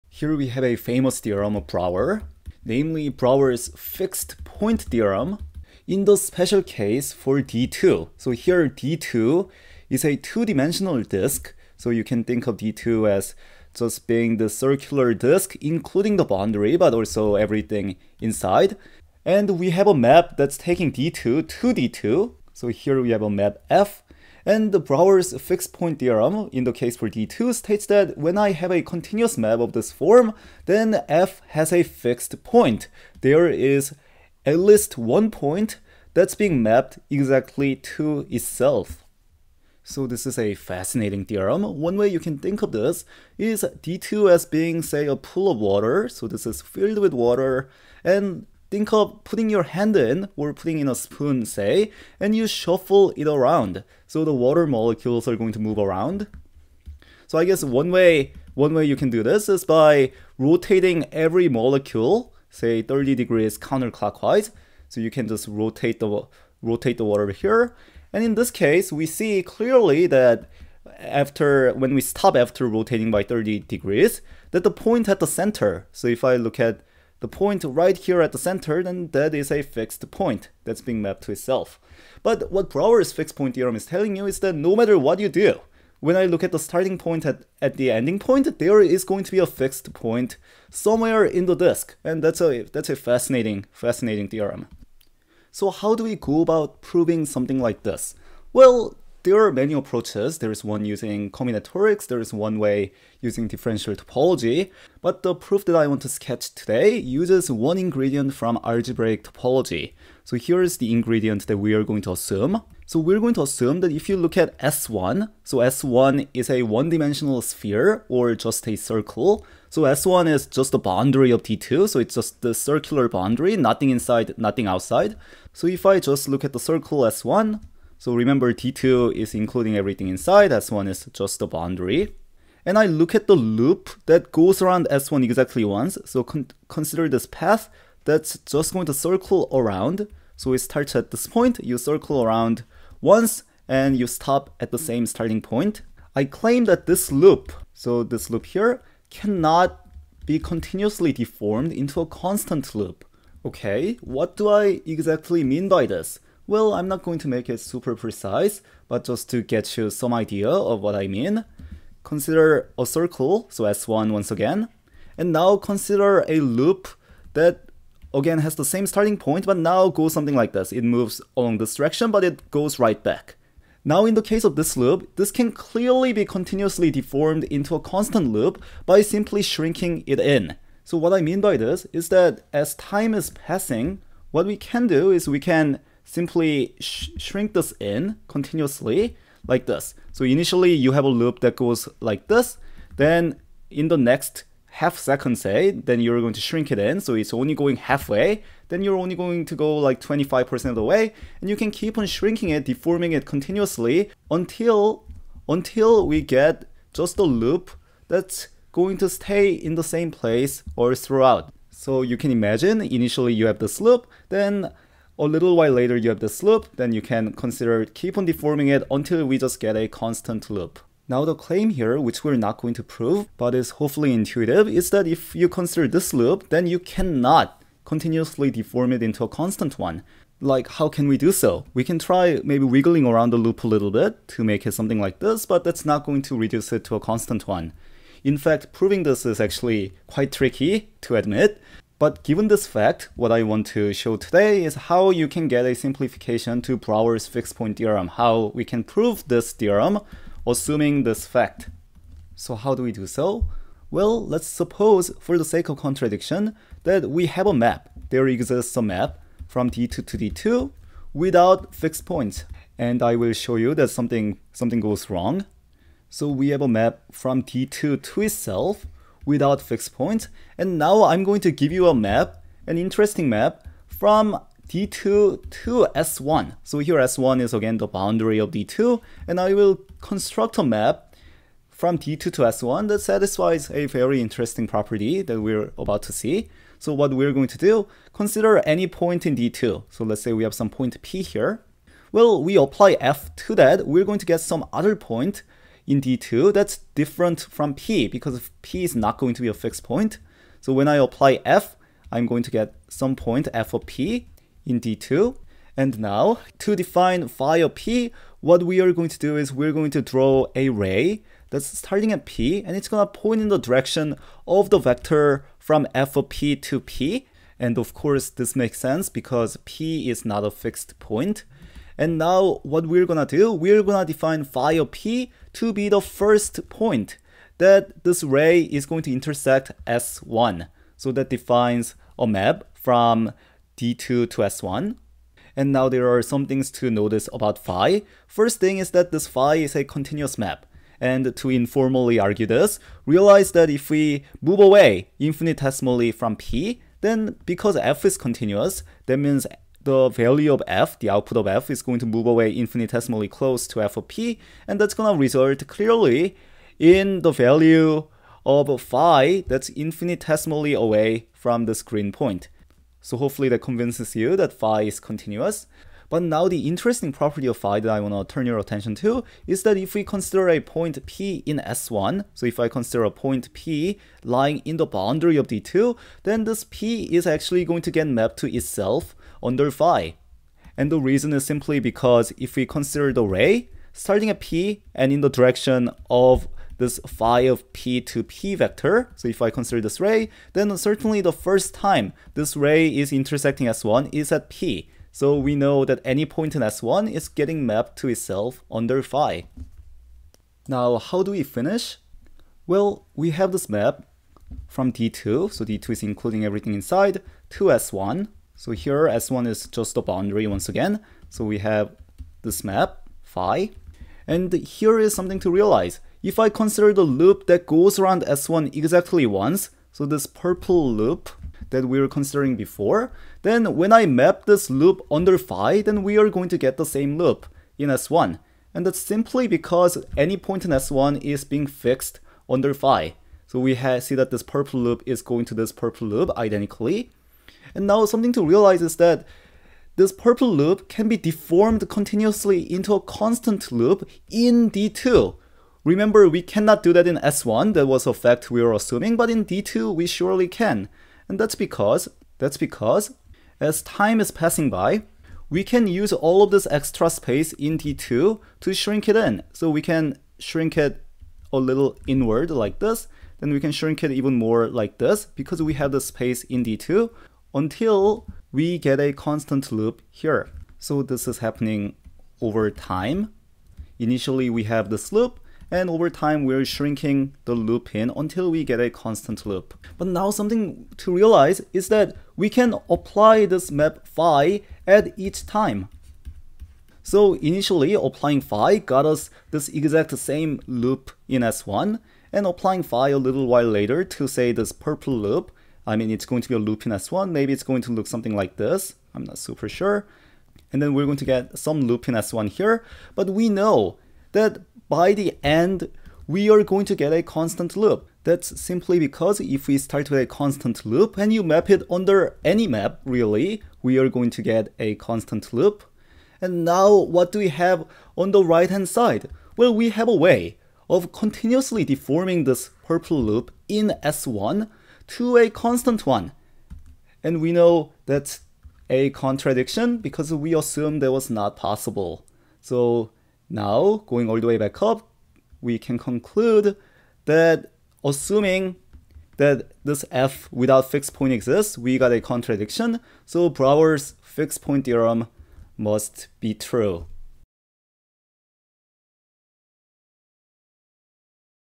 Deformation to a Constant Loop: Brouwer's Fixed Point Theorem for D^2
We give an informal proof of the Brouwer's Fixed Point Theorem for a two-dimensional disc, using the fact that the identity loop of S^1 is not nullhomotopic (i.e., cannot be continuously deformed to a constant loop).

Any likes, subscriptions, comments, and constructive criticisms are very much appreciated.

A proof of this exact flavor with differential topology ingredients can be found in Guillemin/Pollack's "Differential Topology" and Milnor's "Topology from the Differentiabe Viewpoint". A proof using the fundamental group can be found in Munkres's "Topology" and Hatcher's "Algebraic Topology".